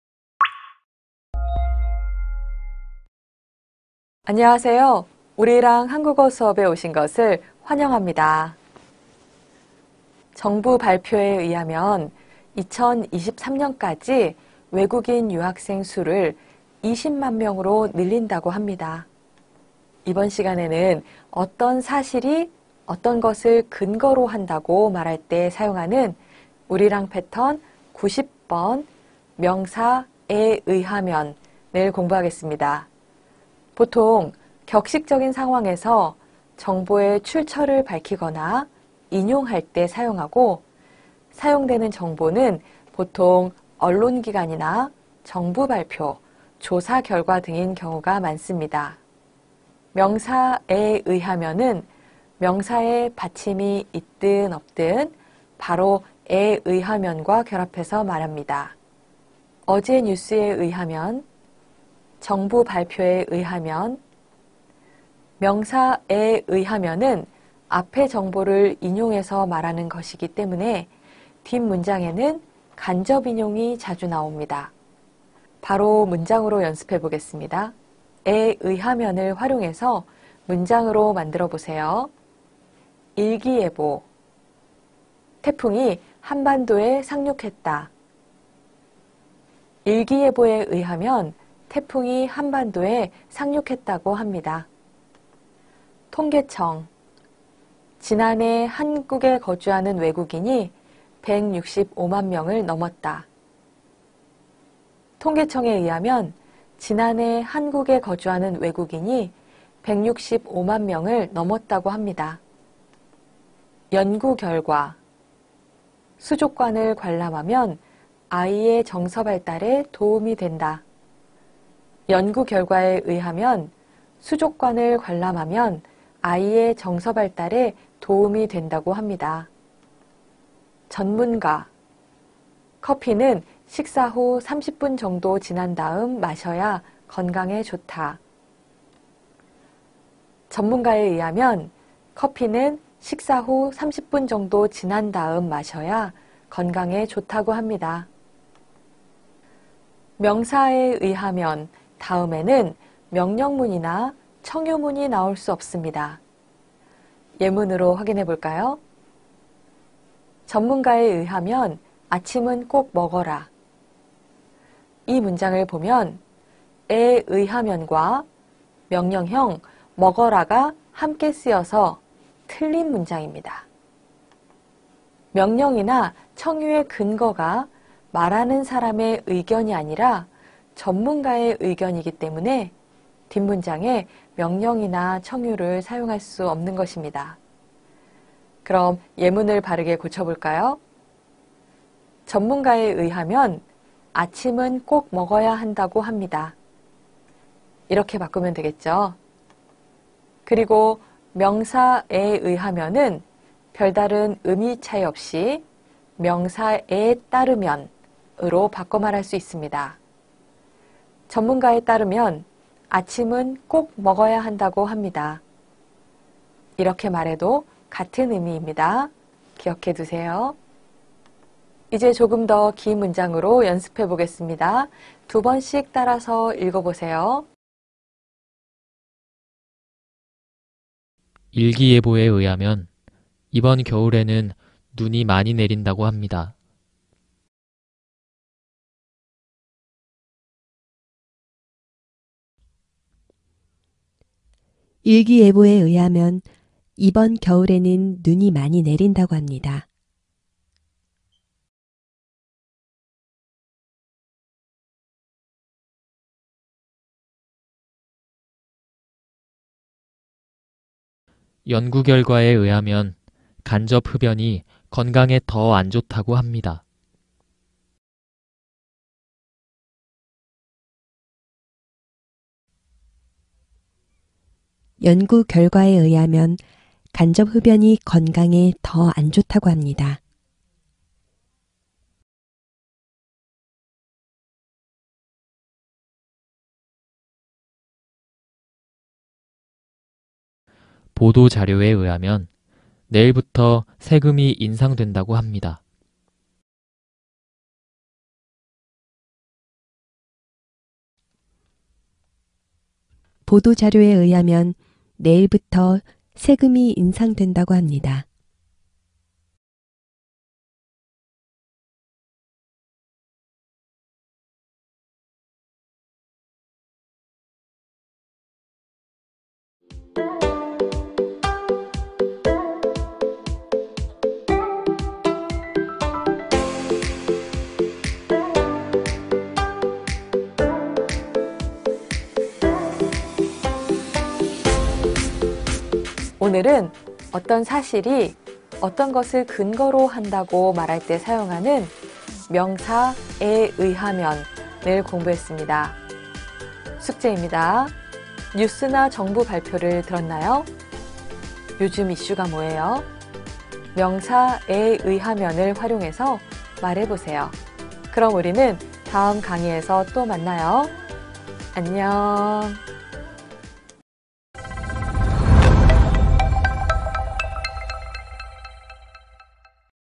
【ENG sub | 100 Essential Korean Grammar】 90. N에 의하면 | TOPIK Lesson | Woorirang Pattern 100
😃 Let's study Korean with Woorirang Pattern 100 lecture🎉👏👏

Today, we are going to study Pattern no.90 [ N에 의하면 ].

90. N에 의하면  [ 인용, 전달 ]

- 어떤 사실이 어떤 것을 근거로 한다고 말할 때
- 보통 정보의 출처를 밝히거나 인용할 때 사용
This pattern is used when you speak about a fact based on something.
Generally, it is used when you reveal the source of information or when you quote.

예문) 정부 발표에 의하면 2023년까지 외국인 유학생의 수를 20만 명으로 늘린다고 합니다.
After class, you can review the grammar with [KPOPx우리랑패턴] video on the channel playlist.